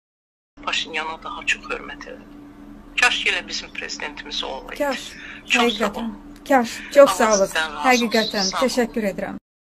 Sevinc Osmanqızı : “Kaş bizim prezidentimiz Paşinyan olardı”
Bəyəndiyiniz videoları paylaşaraq, kanalımıza abunə olaraq dəstək ola bilərsiz. Xoş seyirlər